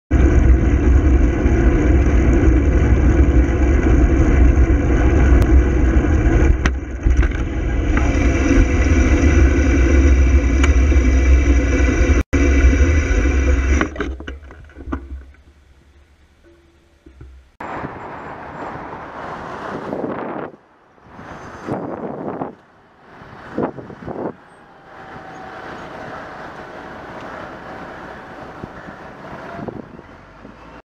FE56JXW pointless
Light ahead is red, traffic has started queueing so it must be time to overtake the cyclist. Should have gone to specsavers.

Video files were split so I've edited them back together but there's a bit of a jump where I did this.

Monday 18th May 2015 approx 17:15